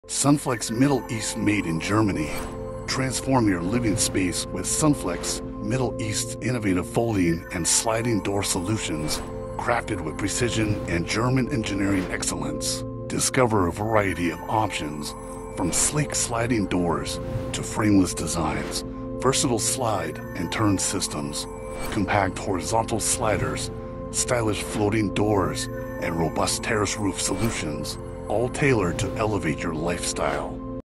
Transform your living space with Sunflex Middle East's innovative folding and sliding door solutions
Sunflex Middle East made in Germany .Transform your living space with Sunflex Middle East's innovative folding and sliding door solutions, crafted with precision and German engineering excellence. Discover a variety of options, from sleek sliding doors to frameless designs, versatile slide and turn systems, compact horizontal sliders, stylish floating doors, and robust terrace roof solutions, all tailored to elevate your lifestyle. Sunflex Middle East is a leading expert in creating top-tier thermal folding doors, recognized for our unrivaled craftsmanship.
Explore our extensive product line featuring minimal sliding, frameless sliding, slide and turn, horizontal sliding doors, floating sliding options, and terrace roof systems. Our products not only deliver exceptional energy efficiency but also offer advanced security features up to resistance class 2 (RC2) and RC1, ensuring optimal protection. Each product is meticulously crafted in Germany to guarantee quality and reliability.